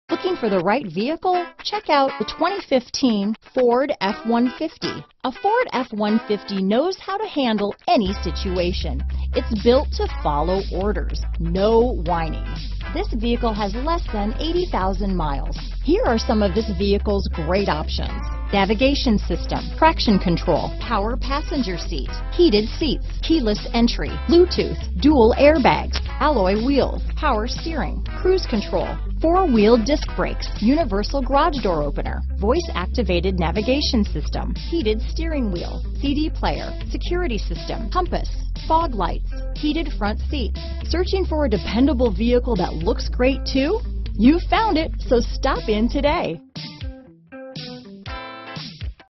2015 Ford F-150 Johnson City TN, Kingsport TN, Bristol TN, Knoxville TN, Ashville, NC 190823A
Ingot Silver Metallic Used 2015 Ford F-150 available in Johnson City, Tennessee at Johnson City Ford. Servicing the Kingsport TN, Bristol TN, Knoxville TN, Ashville, NC area.